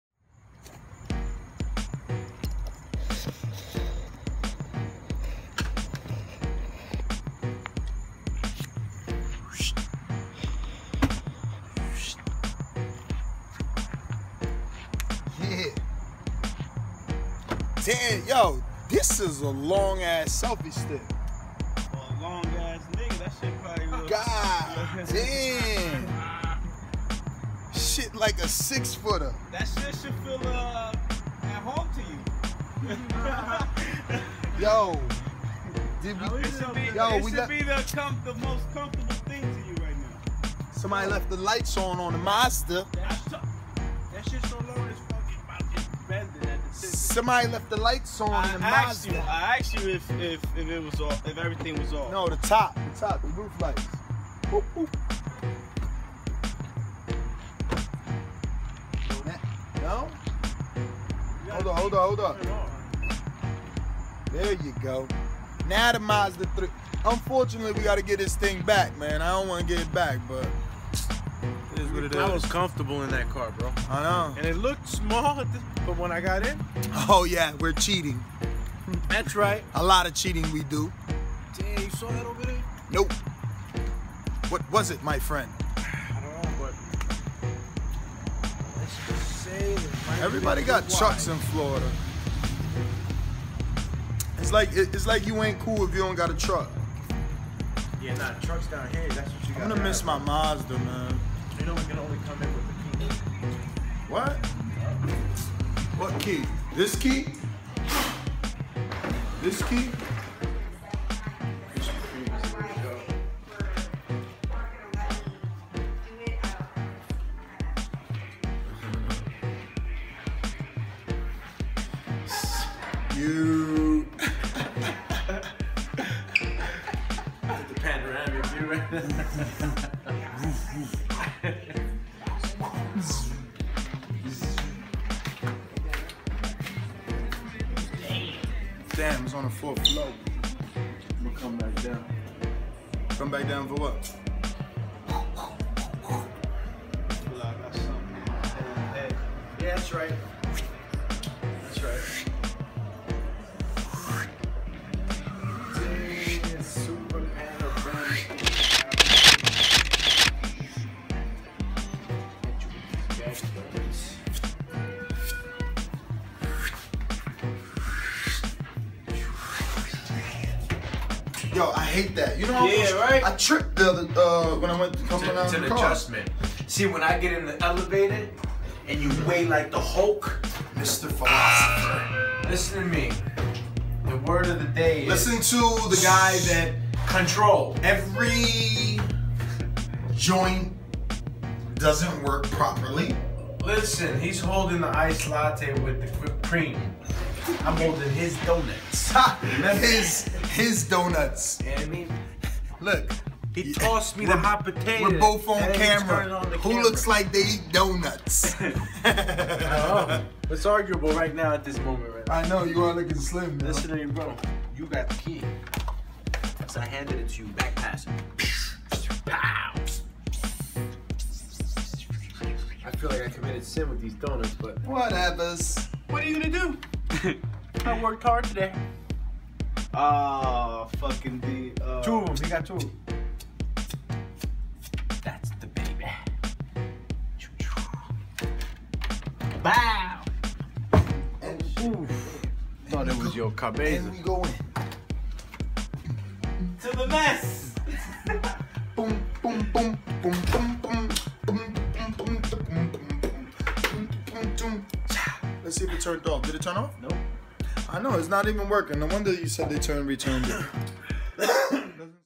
Keith Thurman Road Trip: Mazda 3 Late Night Munchies Run
Nestor and Alex hit the road to make sure they bring the world the best possible coverage. Keith Thurman Road Trip: Mazda 3 Lat Night Munchies Run.